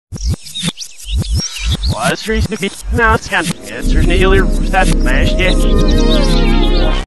a strang man appears in front of me, says some strange, twisted words
heck

Me in the

A